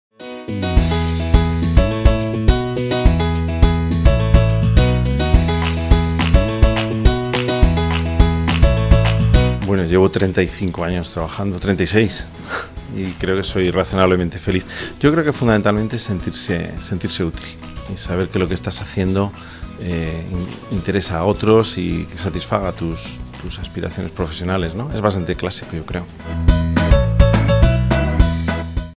El momento Feliz de Alfonso Bilbao
Alfonso Bilbao, Presidente de la Comisión Técnica de la Fundación ESYS nos explica su momento de felicidad en el trabajo.